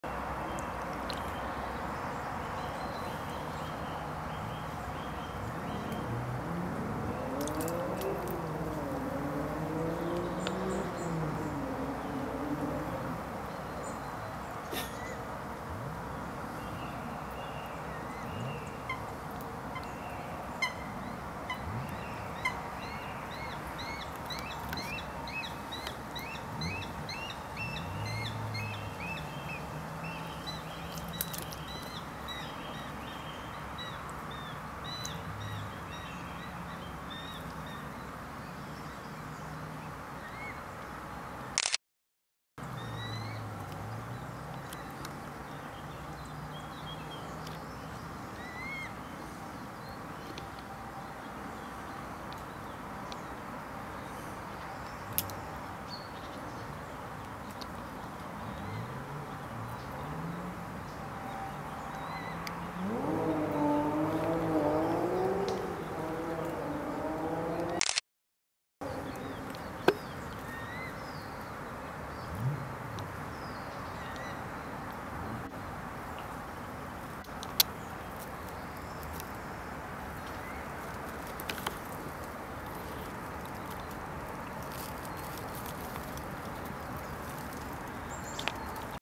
Ringelnatter im Kampf mit Ihrer Beute
Ringelnatter  versucht sich Ihre Beute den Teichfrosch zurechtzulegen, der Frosch hat keine Chance zu entkommen.